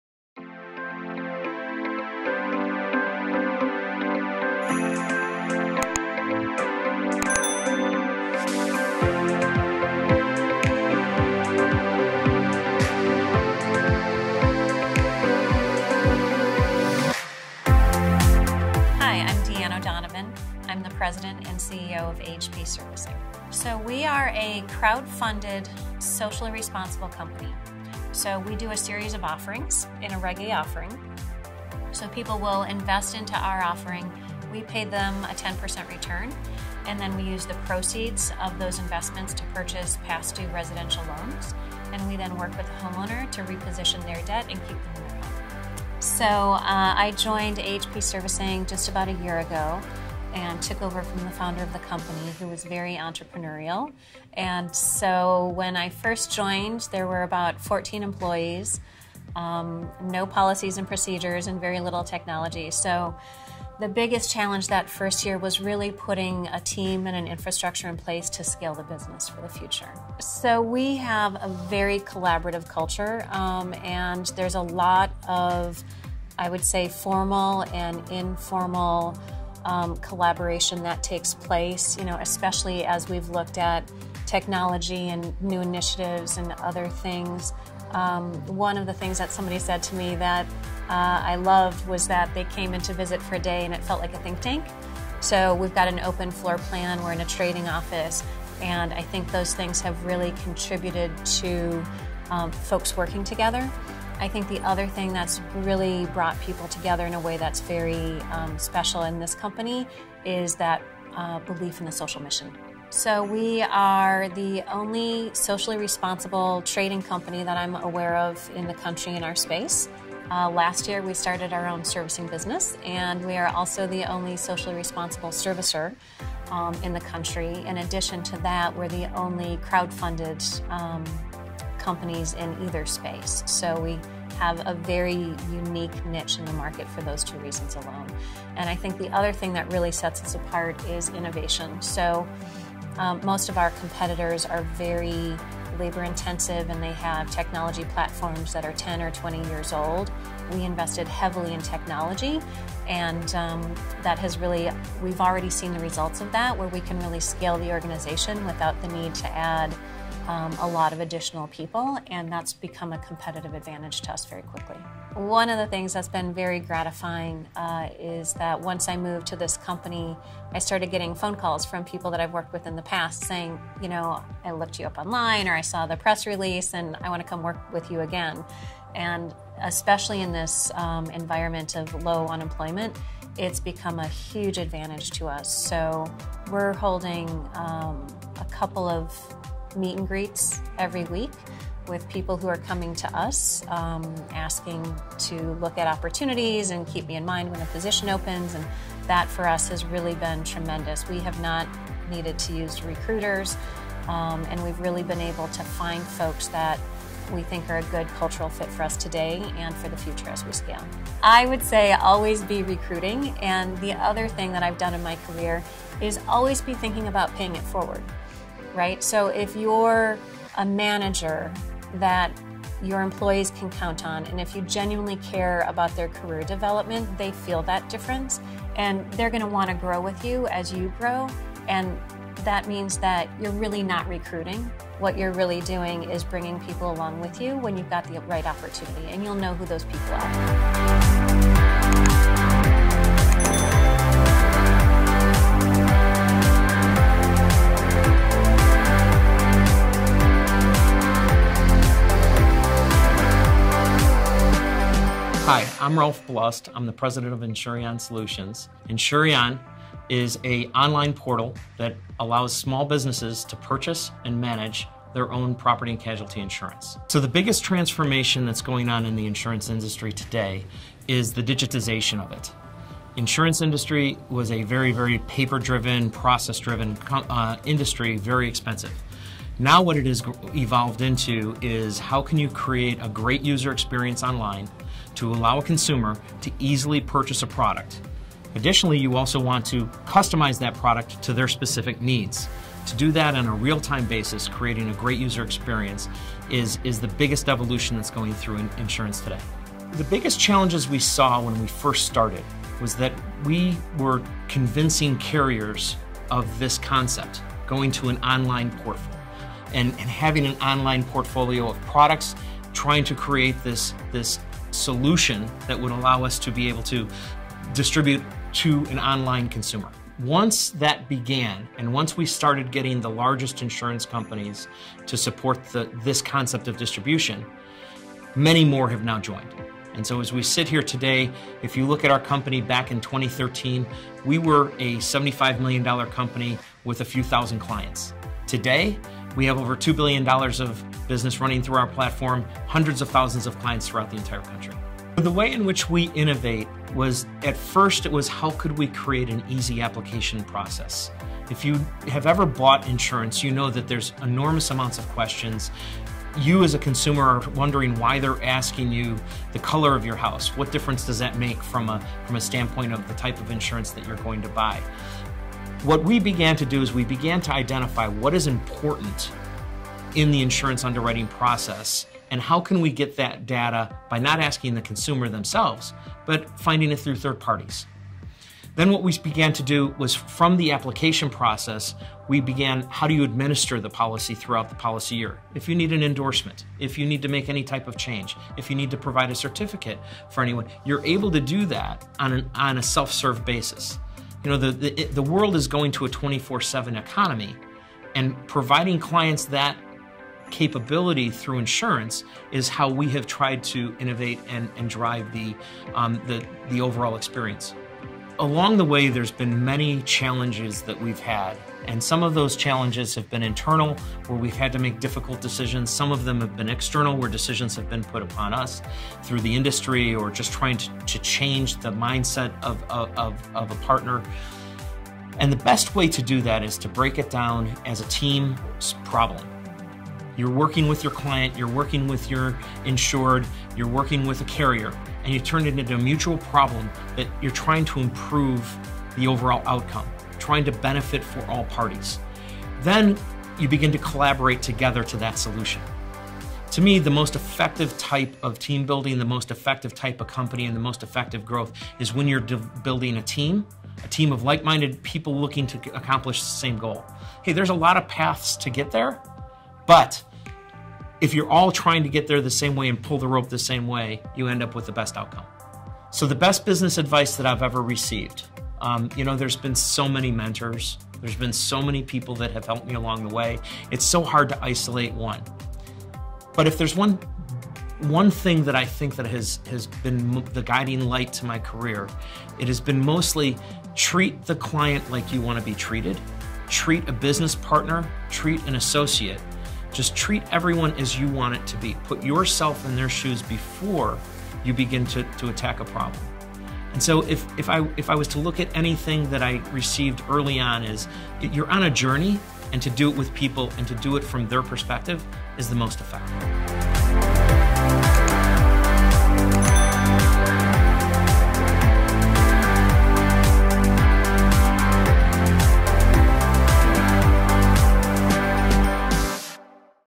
Deann O'Donovan and Ralph Blust
Deann O'Donovan: Real investment. Real impact. These two themes highlight the performance that AHP SERVICING brings to investors and borrowers, as well as the culture and leadership she brings to the oversight of her 21-person team. With more than 20 years of experience leading profit centers, overseeing workout plans and corporate strategy, O’Donovan is primed to change how mortgage servicers conduct their operations. In her role, she oversees the company’s direction, strategy, staffing, and operations for AHP SERVICING, a specialty services of past due residential mortgages.

Ralph Blust is the president of Insureon Solutions, the Wholesale division of Insureon that supports independent insurance agents and small insurance agencies. He also co-leads the B2B operation of Insureon which offers outsourced solutions for the managing of other firms small commercial insurance business. He brings 30+ years of insurance industry leadership experience, including positions with Willis Group as Executive VP and managing partner and leadership tenure at Crump/CRC and AIG. Ralph joined Insurance Noodle in 2010 and became a part of the Insureon team at the time of acquisition. He is a proven leader, known for taking operations with diminishing profitability and making them successful through reengineering from top to bottom. His diverse industry experience equips him with the tools to develop initiatives that improve efficiency and produce revenue growth and organizational effectiveness. Ralphis passionate about boating, fishing and many philanthropic activities.